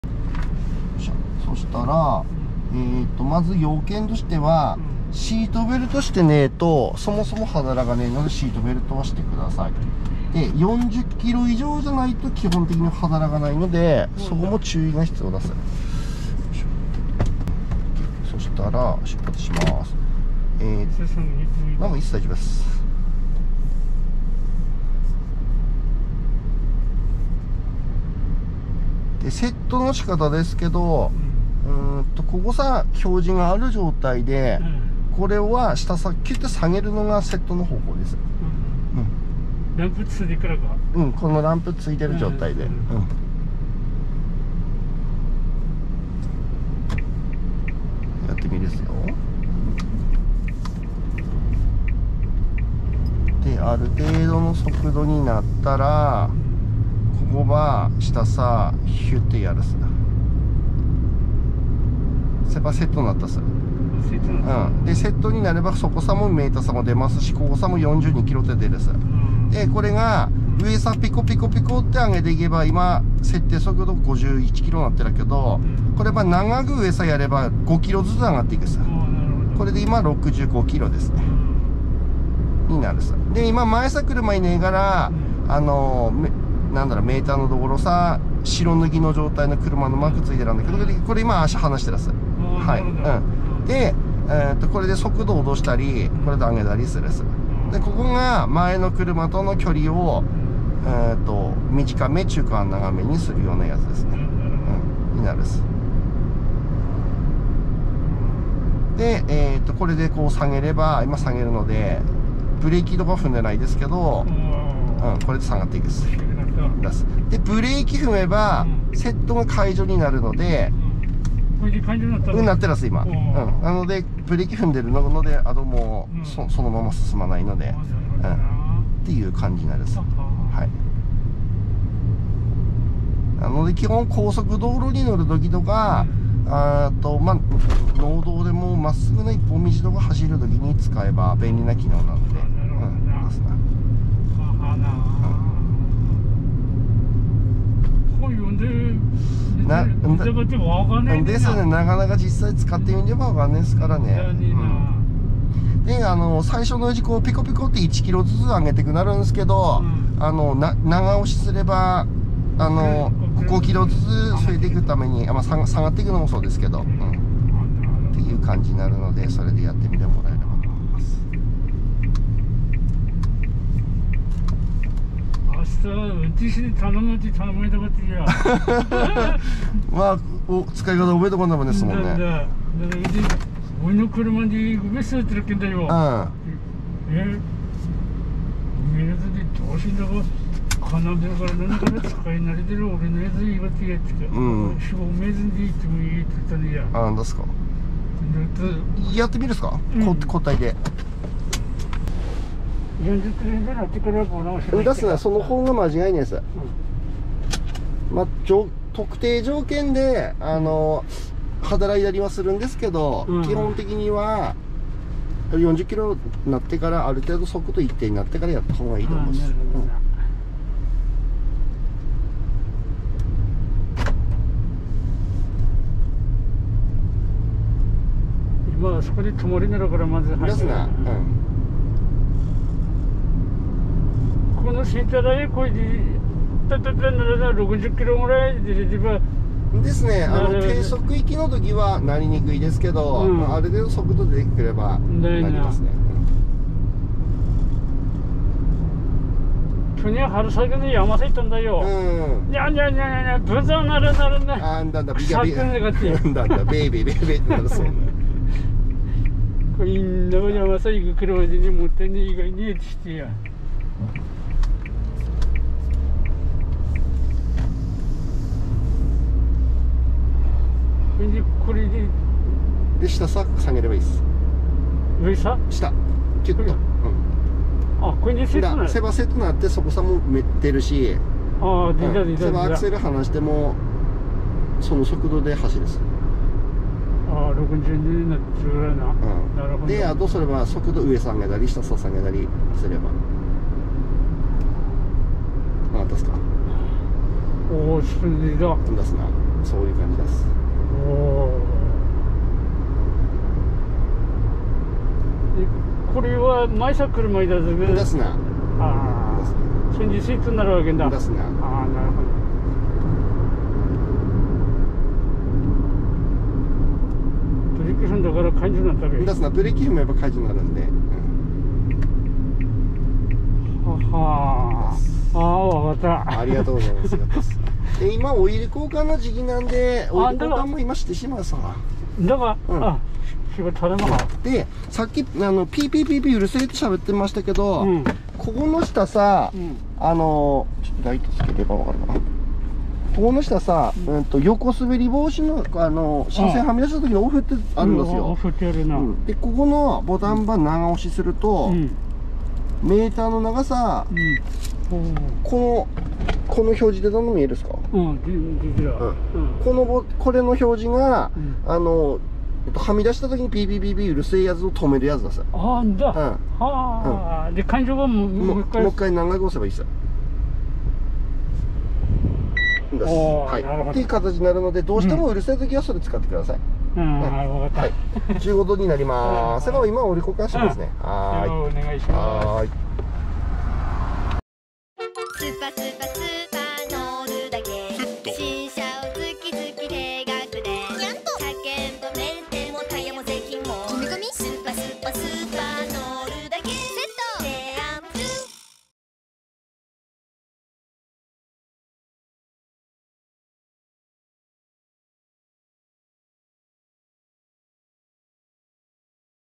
SUZUKIスペーシアカスタムのアダプティブクルーズコントロールはこうやって使おう
クルーズコントロールの機能は
覚えてしまえば簡単ですが
最初はなかなか使いにくいかもしれません
そんなあなたに使い方の解説です。